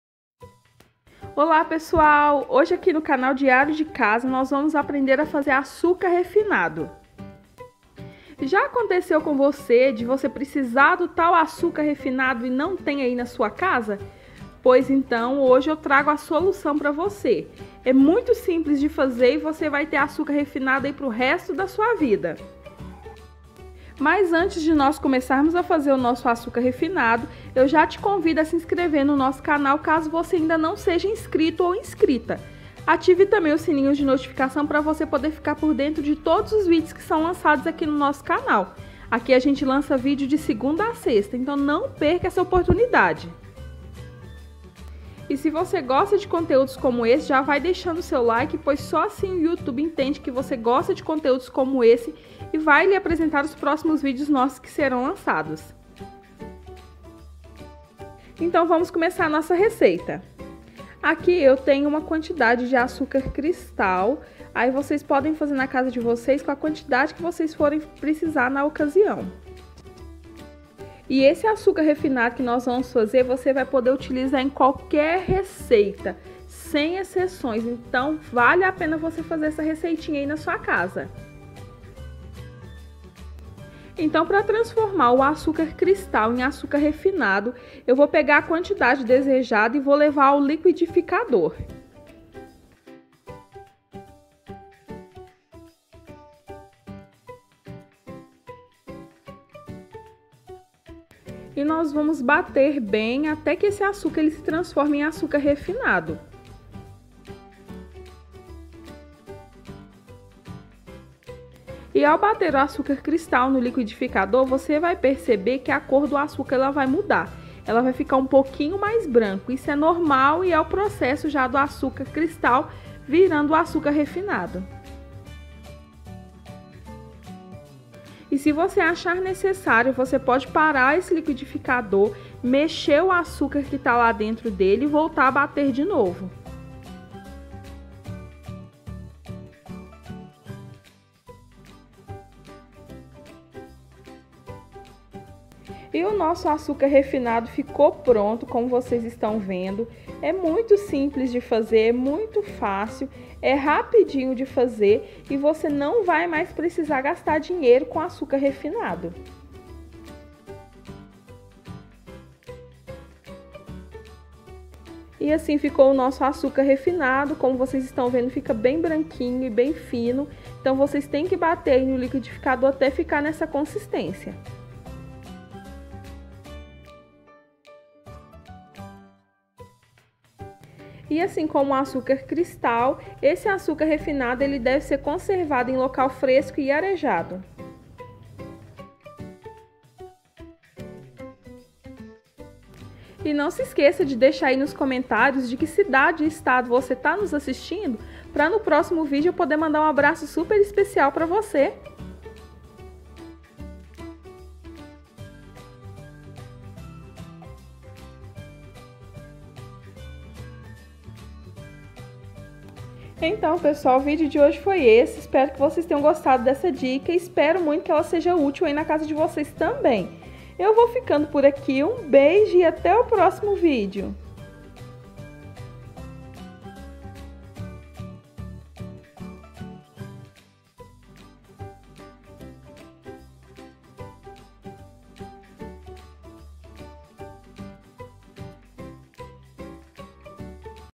🔴 COMO FAZER AÇUCAR REFINADO EM CASA
Olá amores! O vídeo de hoje quero compartilhar com vocês a dica de como transformar açúcar cristal em açúcar refinado. Saiba como fazer açúcar refinado em casa e Não perca essa dica!!

👀MUITO IMPORTANTE➡️✔Comente➡️
👍Para Que o YouTube saiba que você quer receber os meus vídeos
E se tiver alguma sugestão de vídeo é  só deixar nos comentários!!